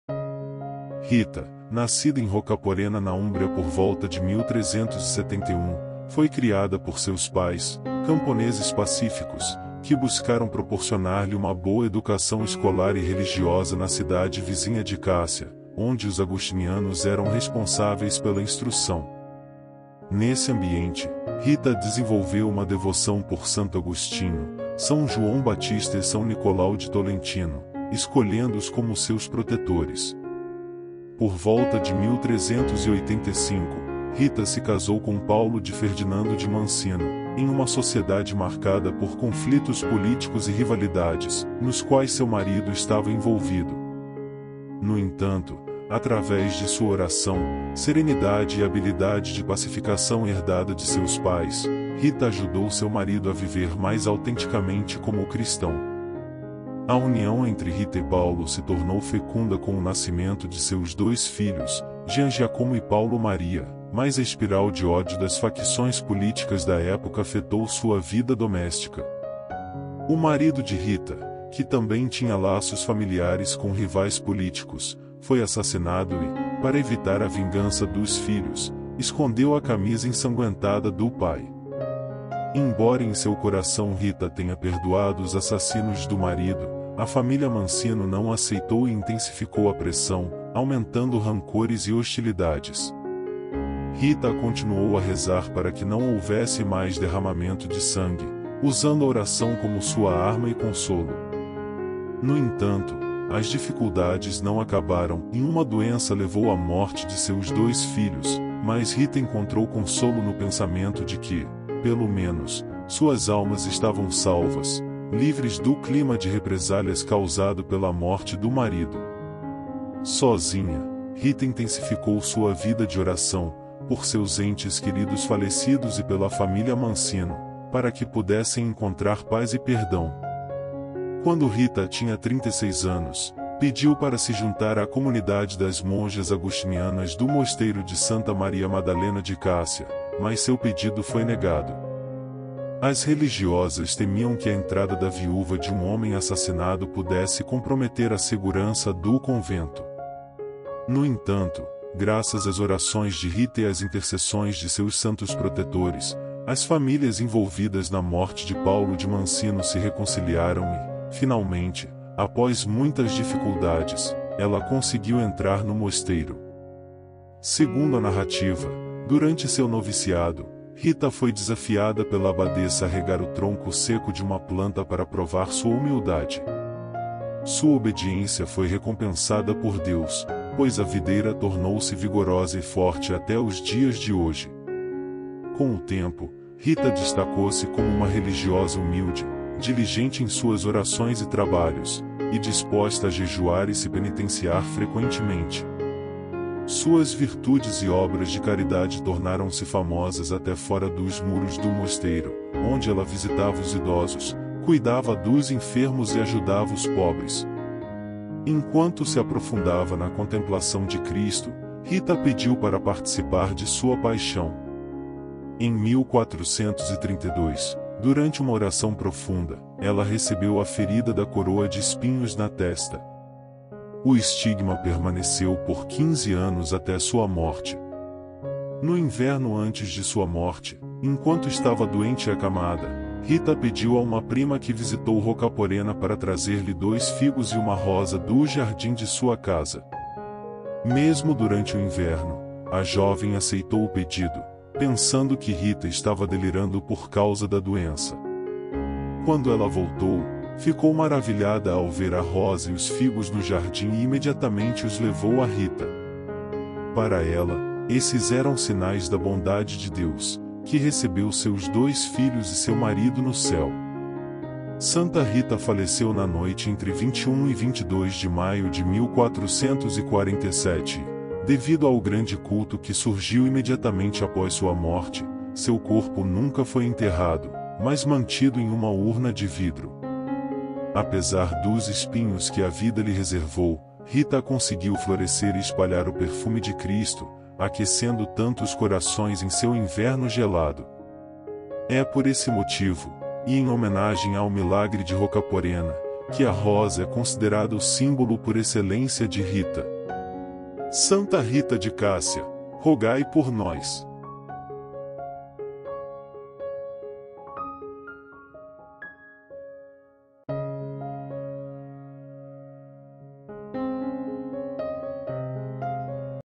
Santo do dia - 22 de Maio - Santa Rita de Cássia
Acompanhe a história do santo do dia Santa Rita de Cássia
🙏 Deus abençoe 🙏

Santo do dia 22 de maio de 2023 - Santa Rita de Cássia
Conheça a história do santo do dia! Não deixe de conhecer a história do santo do dia de hoje. Você vai se surpreender com lindas histórias de vida dos santos.
Hoje dia 22 de Maio , você vai conhecer um pouco sobre a história desse santo do dia de hoje que é muito especial, Santa Rita de Cássia